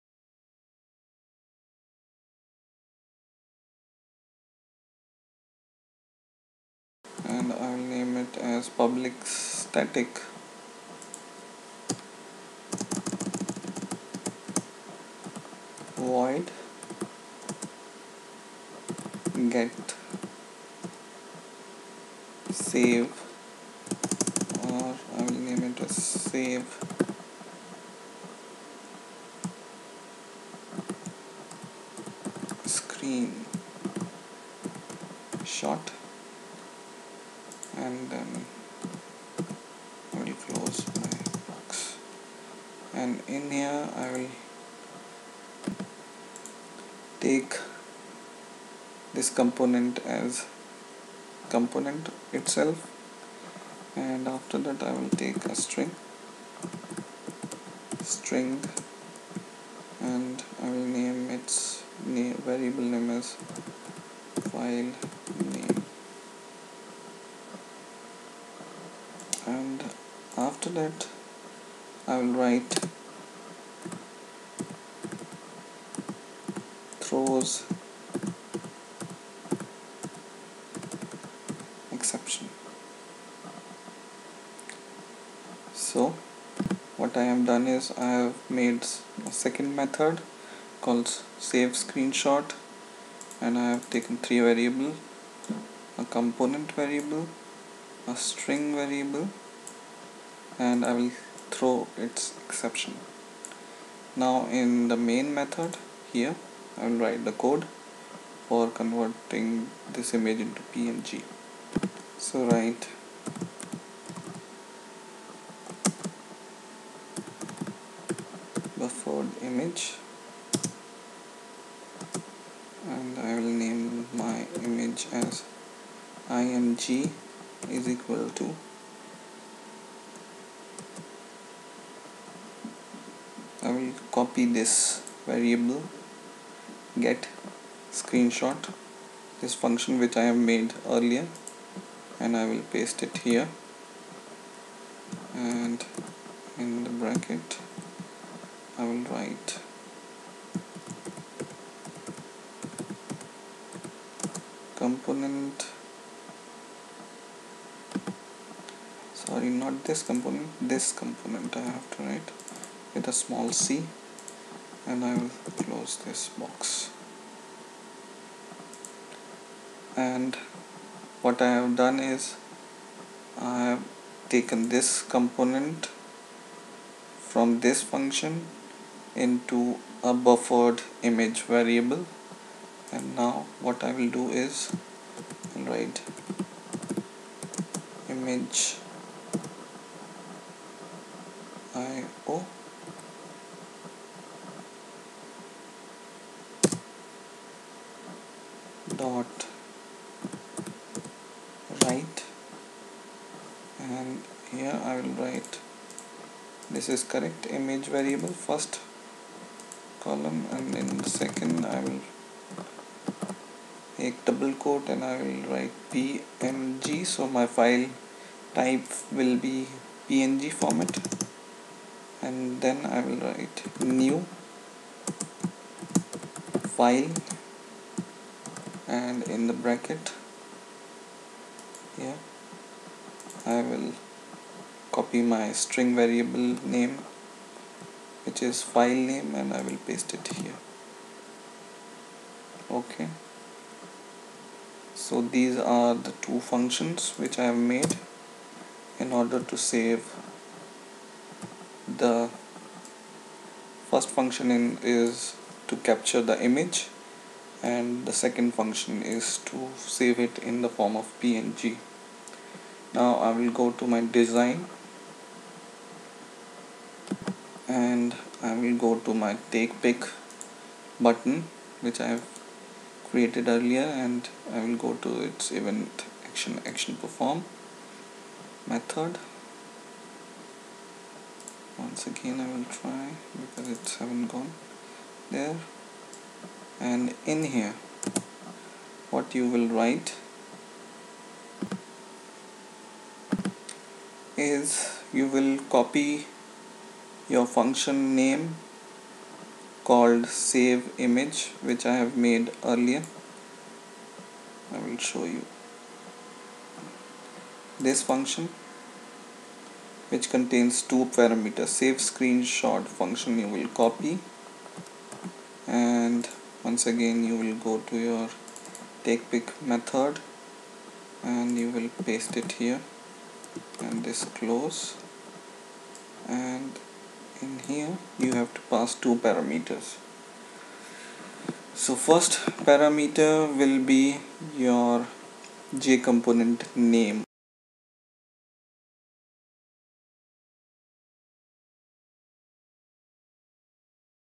Java prog#101. How to Capture or save image of JFrame ,JPanel and swing Component part -3
Java Swing : Obtain `Image` of JFrame
swing - Java - Get an Image of a JPanel
When saving JPanel as image
java - how to save panel as image in swing